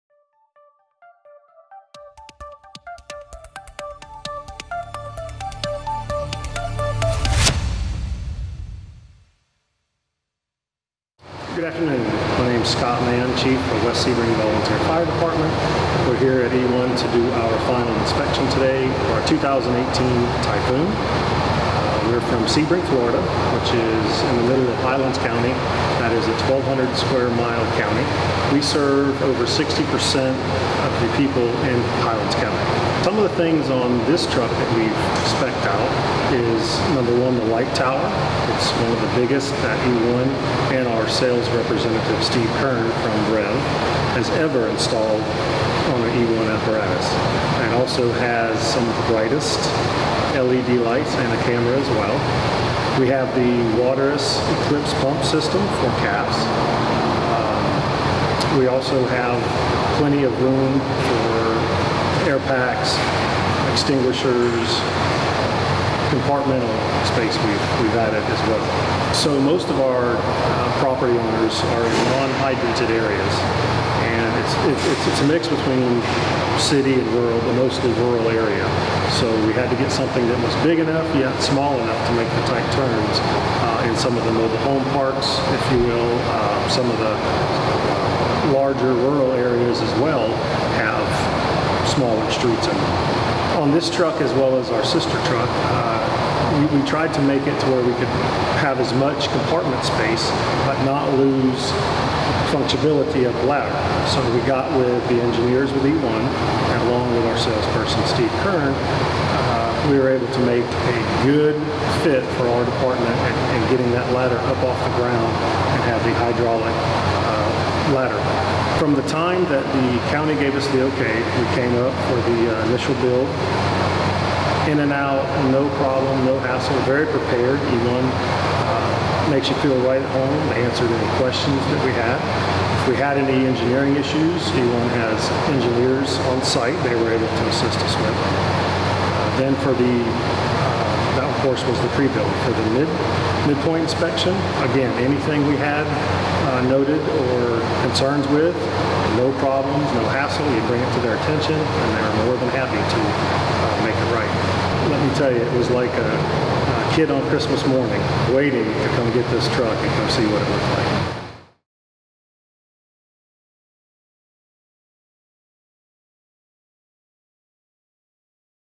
Truck Talk with West Sebring Volunteer Fire Department
Utilizing one of the biggest light towers ever installed on an E-ONE fire truck, the West Sebring Volunteer Fire Department aims to serve their rural area with their new E-ONE Typhoon Pumper. Find out more in this episode of Truck Talk.